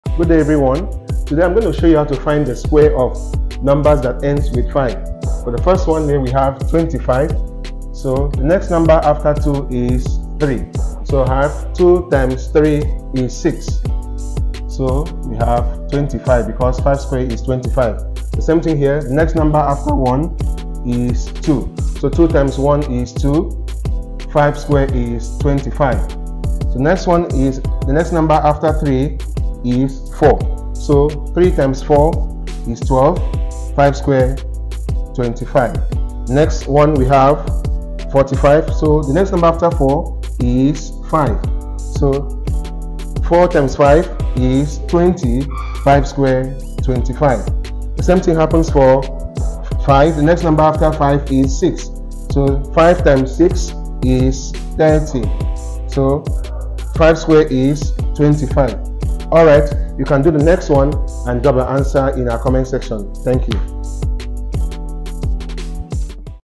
Why Squaring Numbers Ending in 5 is So Easy
Squaring Numbers Ending in 5: The Ultimate Mental Math Hack!

🔢 Unlock the secret to instantly squaring ANY number that ends in 5! This video reveals a mind-blowing trick that will transform your mental math skills and impress everyone around you.

What You'll Discover:
- A foolproof method to square numbers ending in 5
- Why this trick works every single time
- Step-by-step breakdown of the technique
- Real-world applications of this mental math shortcut

The Magic Formula Revealed:
When you want to square a number ending in 5, follow these simple steps:
1. Multiply the first digit by the next whole number
2. Append 25 to the end of the result

mathematics tricks